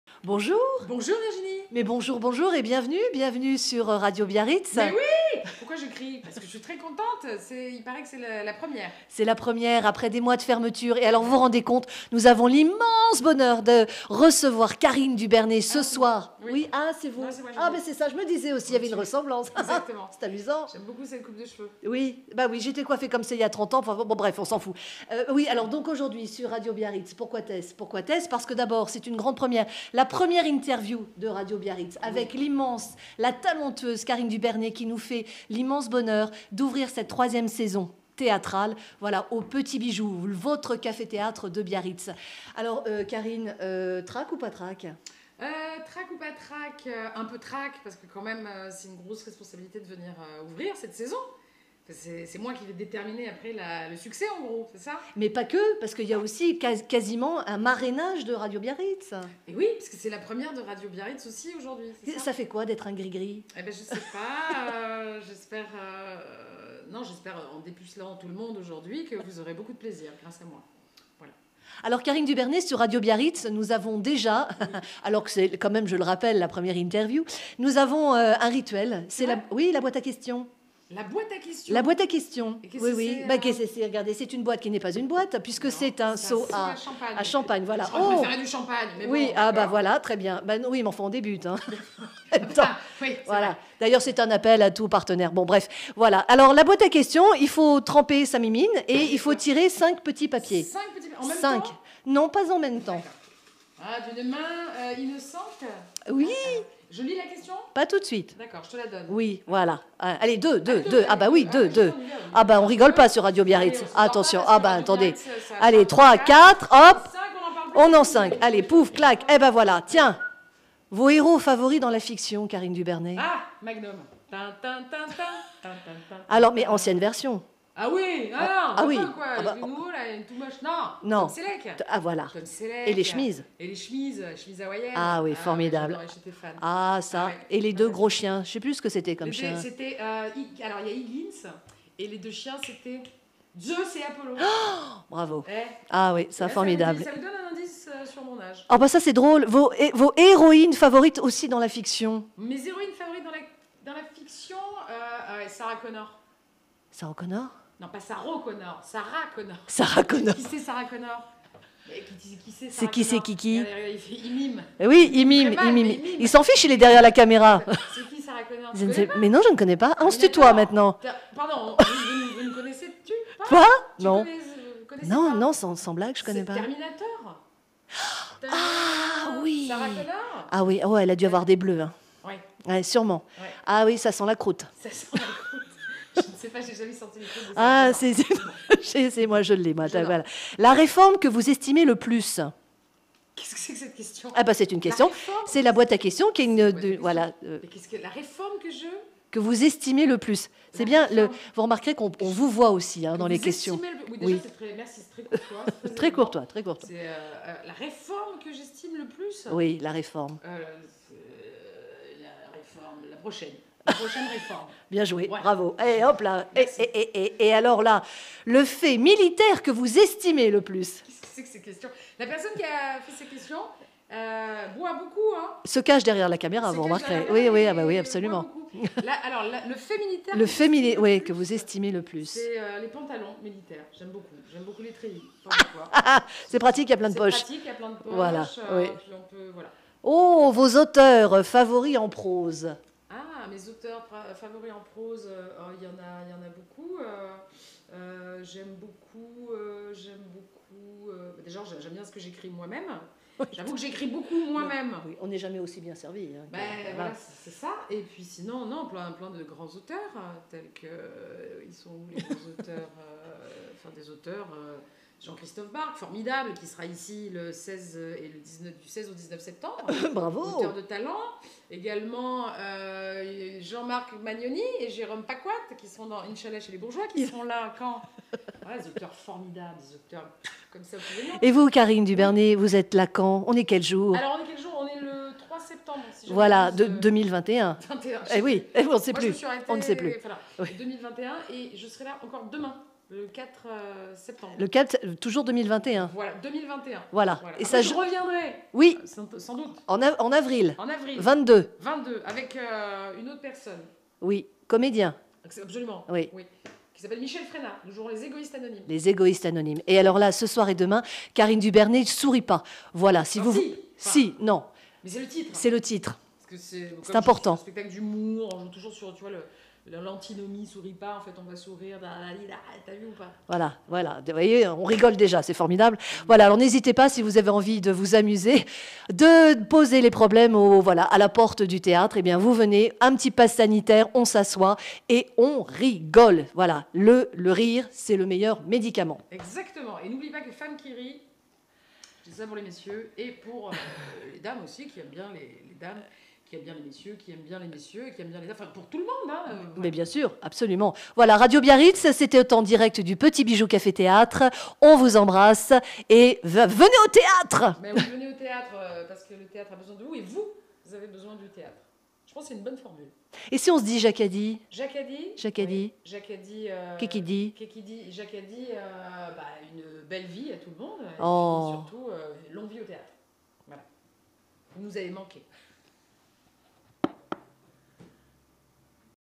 Karine Dubernet - au Petit Bijou Biarritz - face à notre reporter de charme : Virginie Stevenoot
Grande rentrée théâtrale pour Le Petit Bijou Biarritz. Virginie et Mehdi nos hôtes, nous offre sur une plancha (= plateau pour les non initiés ;-)) notre première interview et par la même occasion une magnifique marraine à Radio Biarritz.

❤️ Karine Dubernet (son spectacle : souris pas ! le 3 et le 4 sept 2021) - Quelle belle âme et quel beau sourire communicatif ! Karine se prête au jeu du questionnaire de Proust face à notre reporter de charme : Virginie Stevenoot, à l’occasion de l’ouverture de la saison théâtrale 2021 du Café - Théâtre - Le Petit Bijou à Biarritz.

❤️ Mille mercis à Virginie et Mehdi pour leur accueil et leur amitié.

radio-biarritz.fr